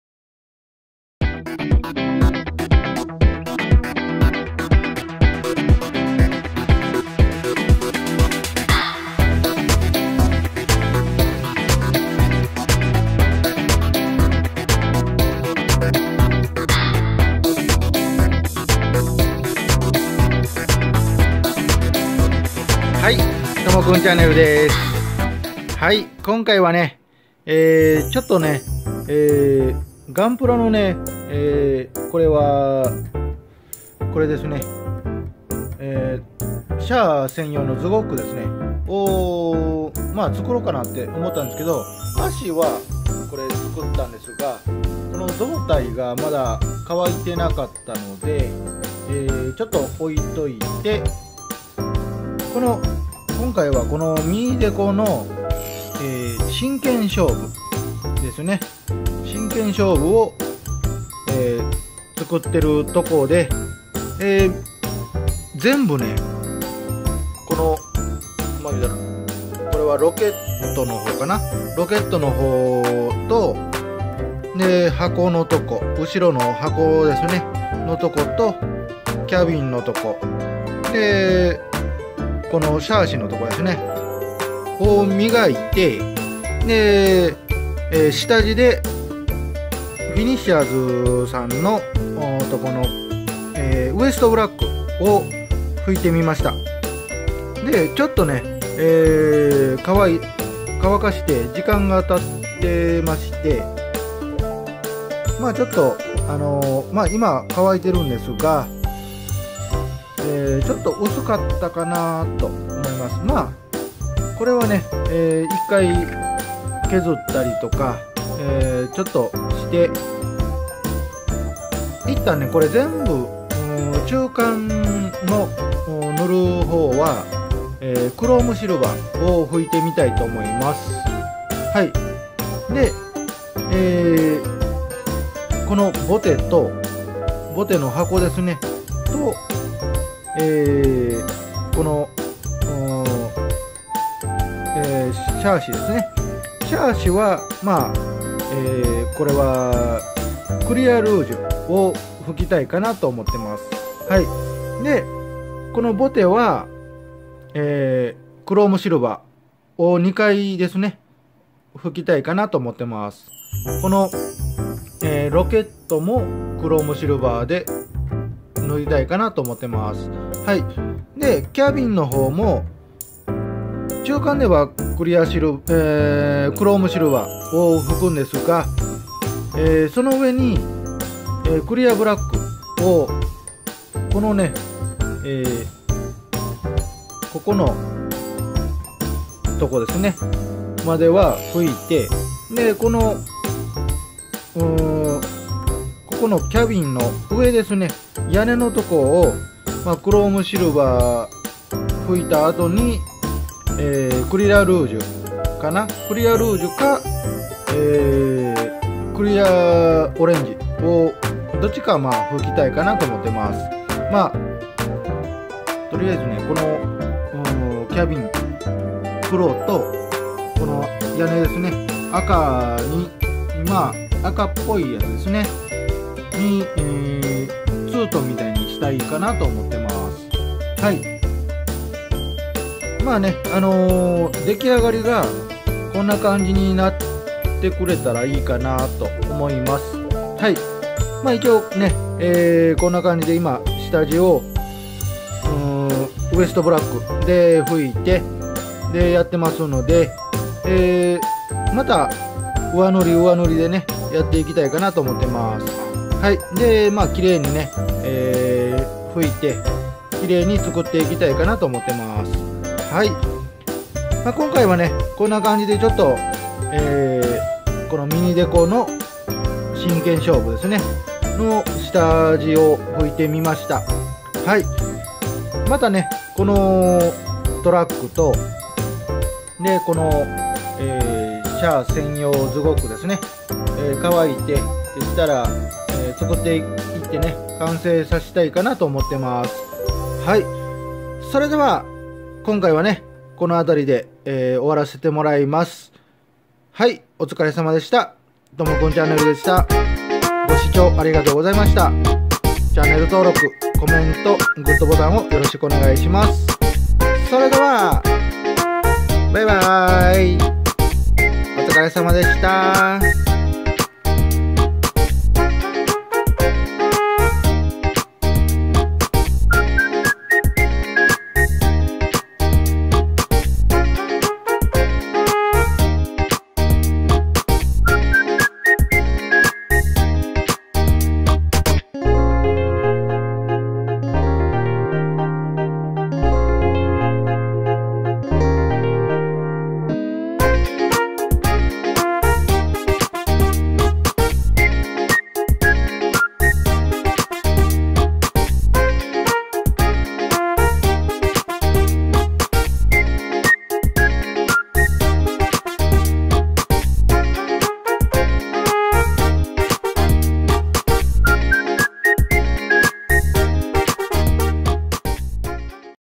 昨夜は動画配信出来ずにすみませんでした🙇今回はミニデコの真剣勝負に下地を吹いて、シャア専用ズゴックの部品を組み立てました😁👏🎨✴
ミニデコの真剣勝負を作っていきますが、下地にフィニッシャーズカラーのウェストブラックを吹いてみました😁👏🎨✴笑笑✴それとシャア専用ズゴックを少し作ってます😁👏またきれいに作って完成させていきたいですね🙆🎉笑笑